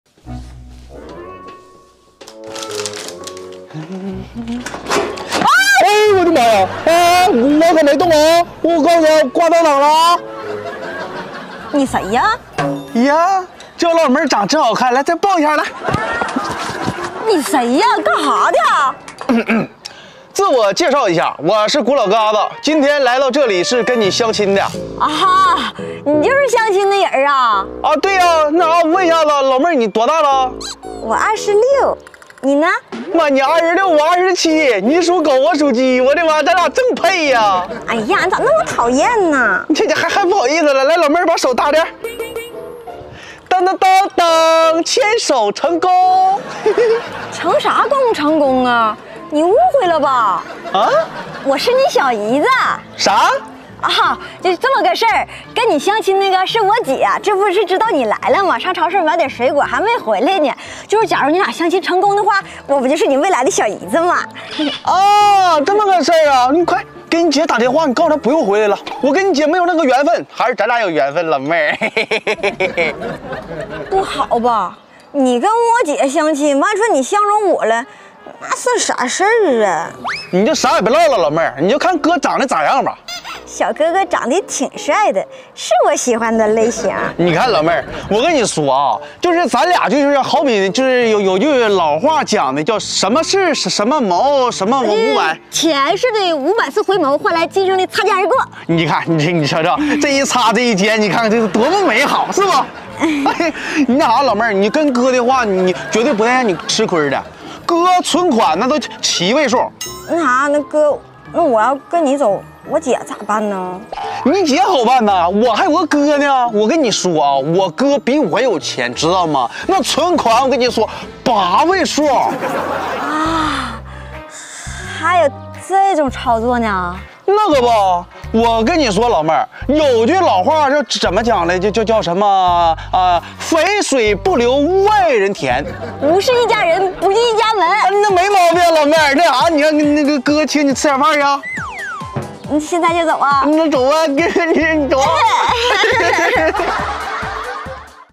男子相亲，错把未来小姨妹当相亲对象，一把将她搂入怀中，太逗了
【谷歌喜劇】女房東來收租,錢沒收到自己反倒貼,給小伙安排明白了